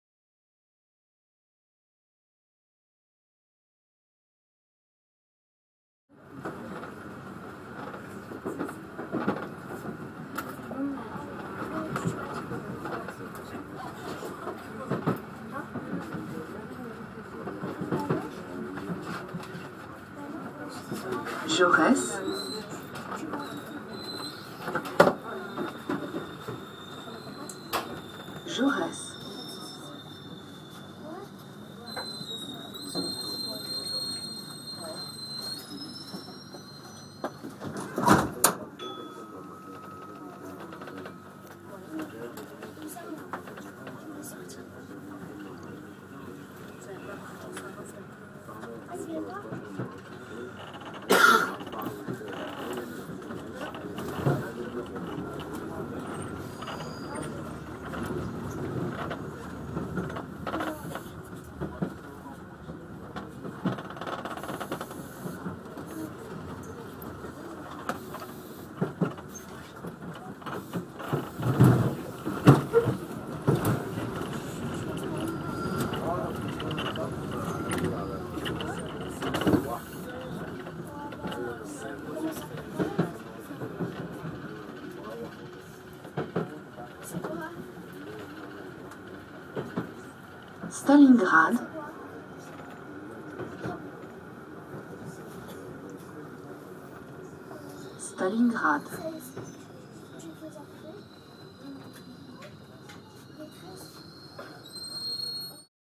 Balade en MF-01 sur la ligne 2
une petite balade en MF-01 sur la ligne 2 du métro parisien, juste avant jaures jusqu'à stalingrad